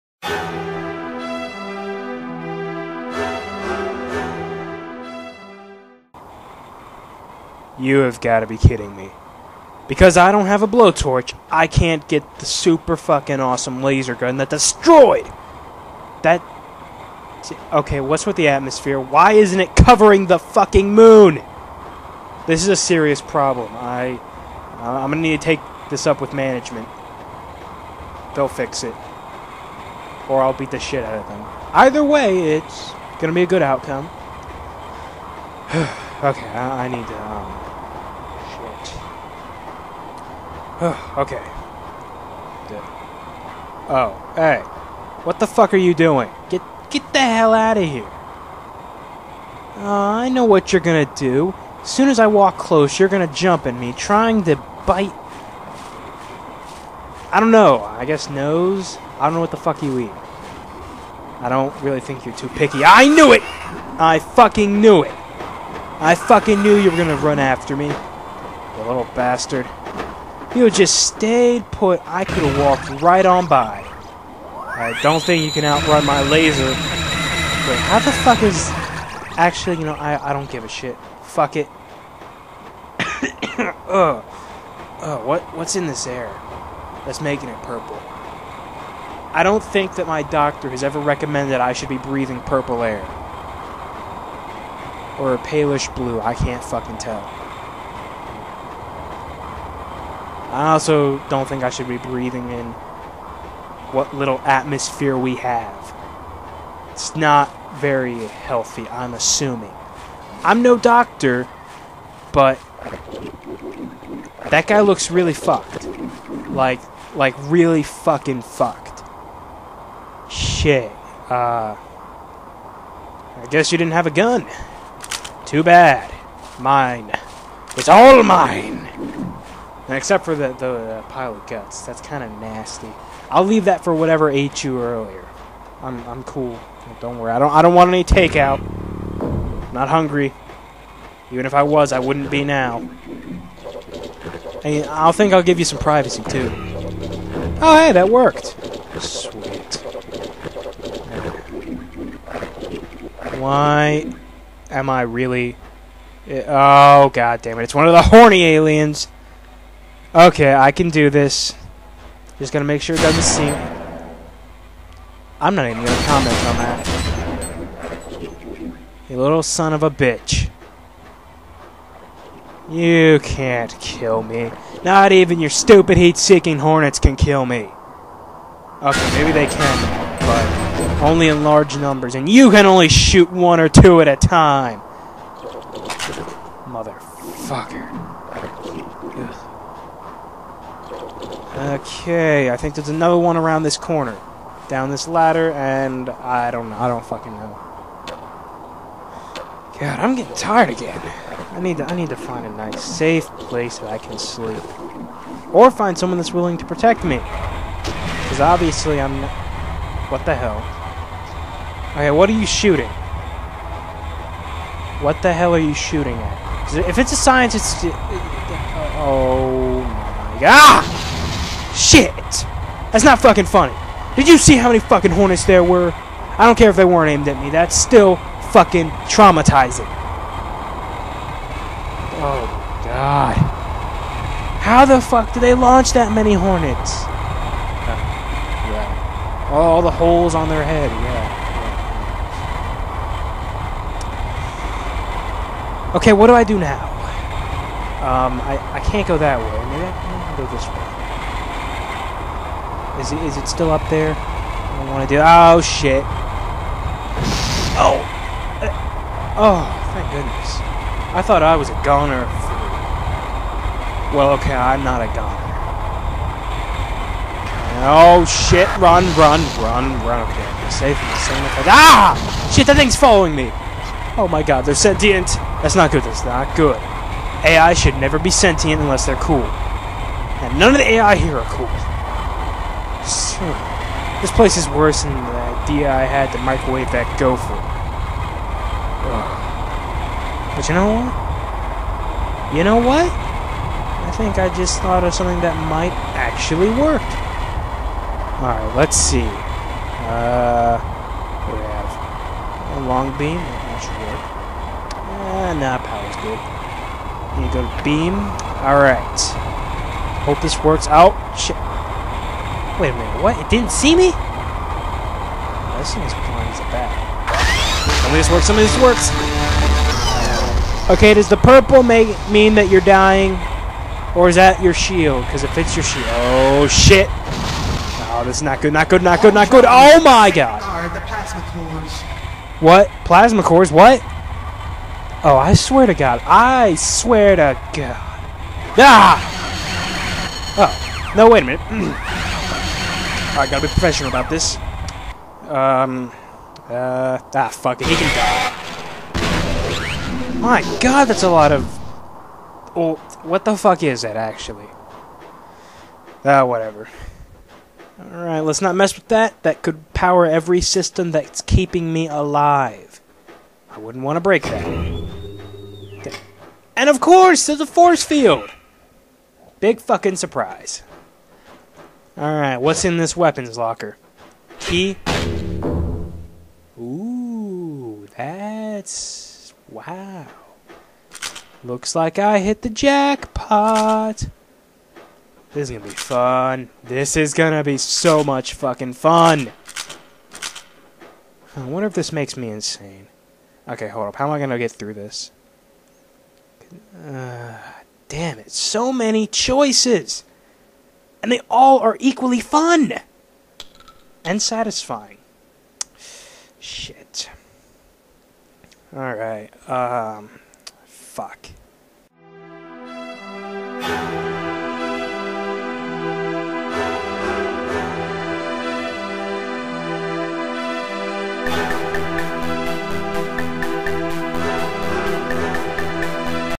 Archer's Mind Episode 15
The inner thoughts of Major Archer, breather of purple and member of G.A.H (Gentlemen Against Hornets). In this episode, our heroic chump finds a corpse (or what's left of it), tries to fight an aerial drone, and comes across a plasma core.

So, here's another interactive bit. Tell me what you think Archer should do to royally fuck up that plasma core he stumbled upon.